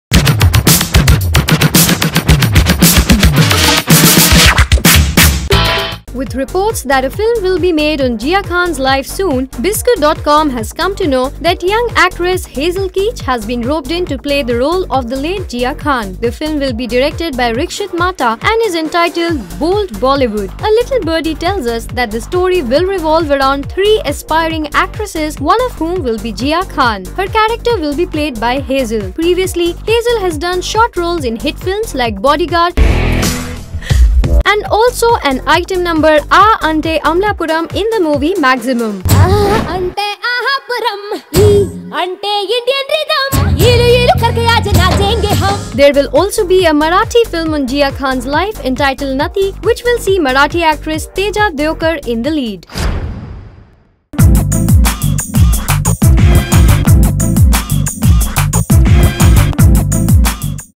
Now, a film on Jiah's life
With reports that a film will be made on Jiah Khan's life soon, Biscoot.com has come to know that young actress Hazel Keech has been roped in to play the role of the late